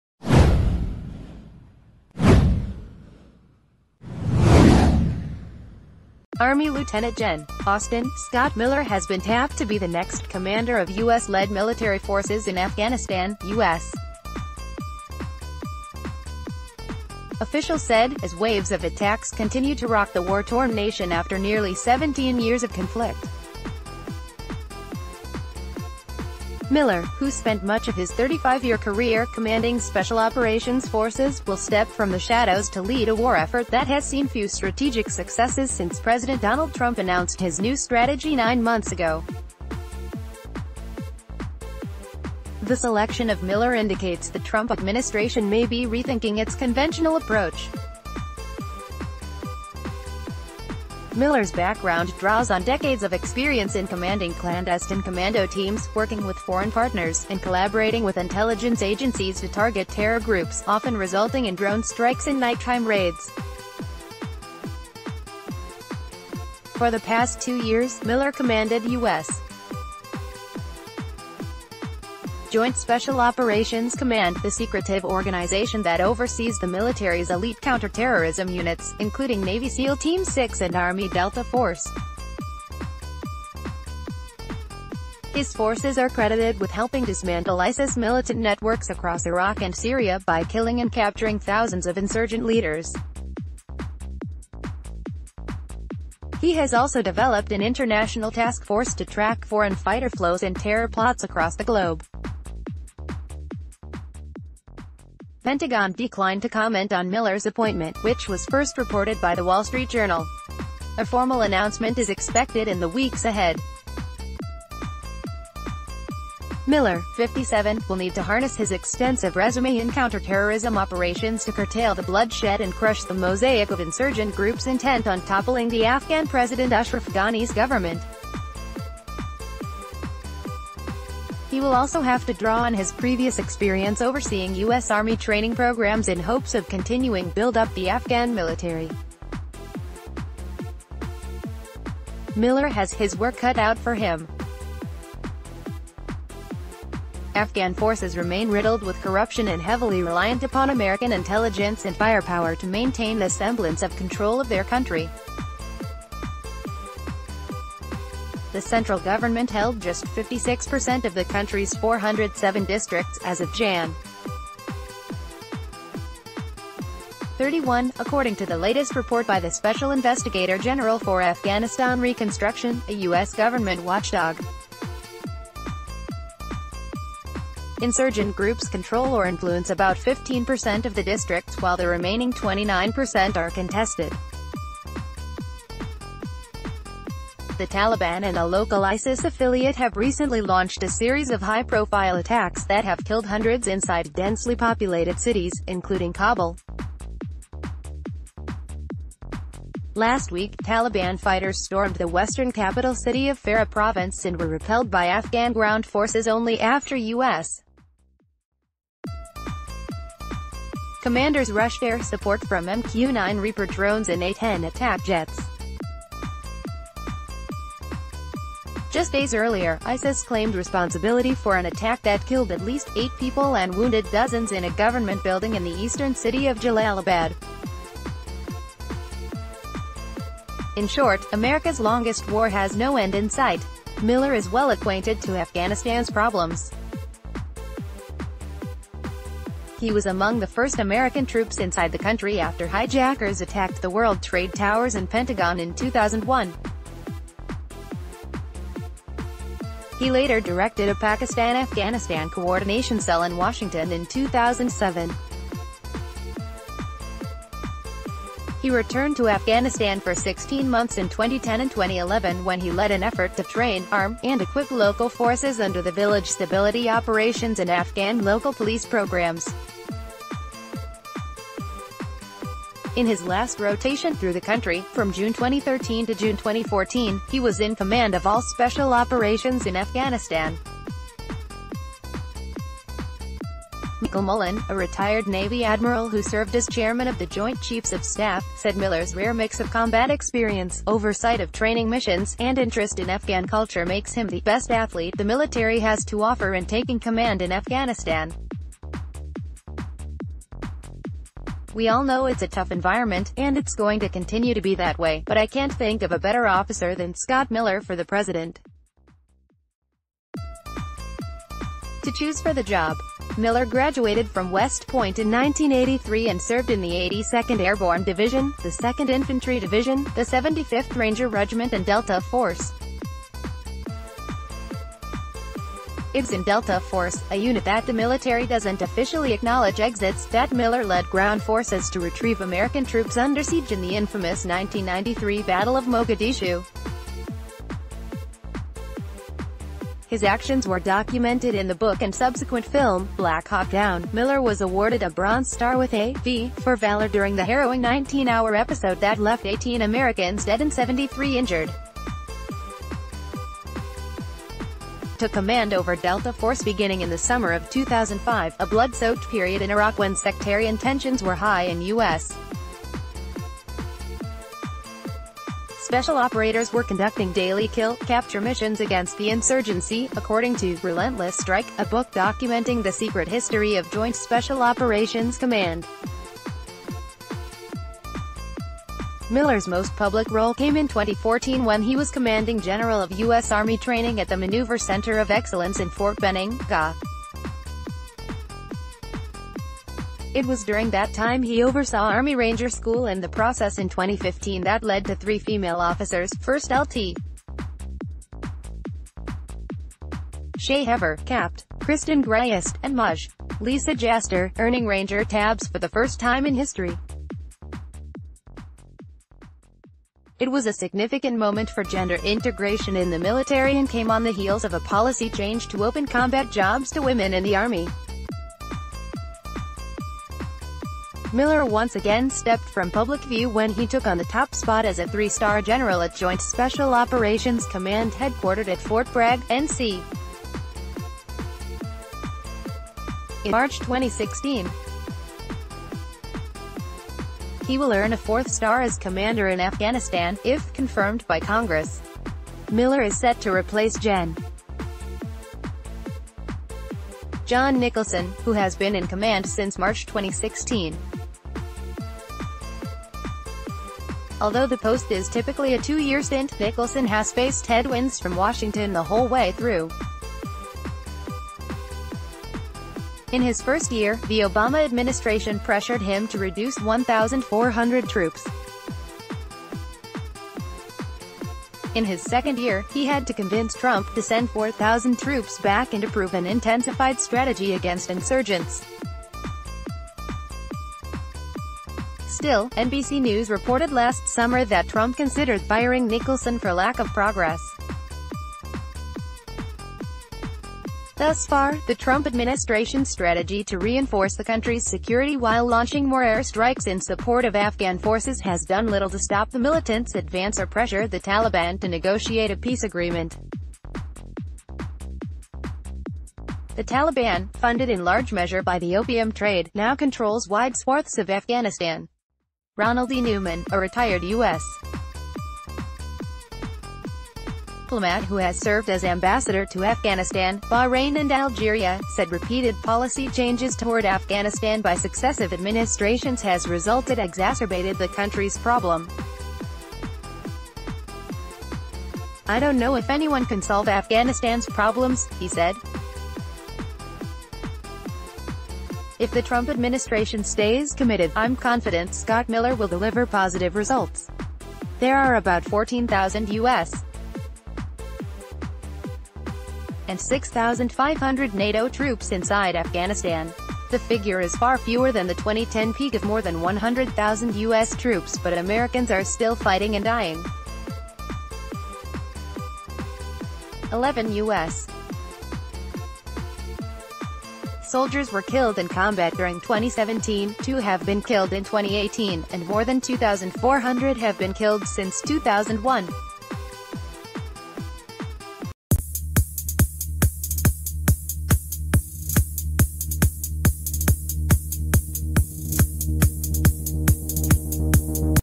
Trump's Pick to Oversee Afghanistan Could Signal a New Approach
Army Lt. Gen. Austin “Scott” Miller has been tapped to be the next commander of U.S.-led military forces in Afghanistan, U.S. officials said, as waves of attacks continue to rock the wartorn nation after nearly 17 years of conflict. Miller, who spent much of his 35-year career commanding Special Operations forces, will step from the shadows to lead a war effort that has seen few strategic successes since President Donald Trump announced his new strategy nine months ago. The selection of Miller i...